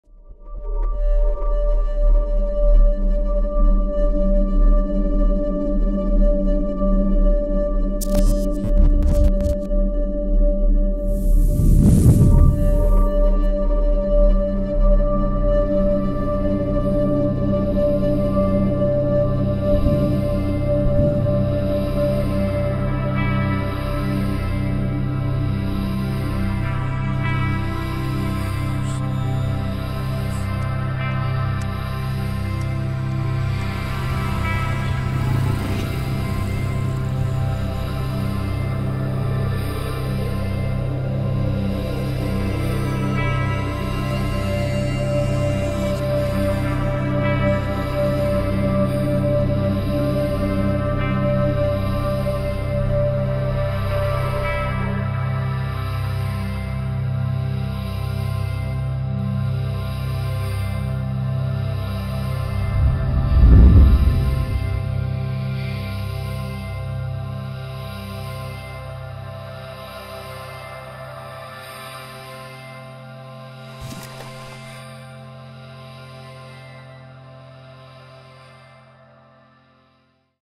Abstract // fashion-art
I had a chance to shoot a random Scenes.
Here is some mixed of the B-roll I captured. Aerial, and some abstract shoots!

Gear
Canon 6D
Lens 20mm USM f2.8
magic lantern raw video

Directed-filmed-sound designed: sergoxo
Model: MahThhan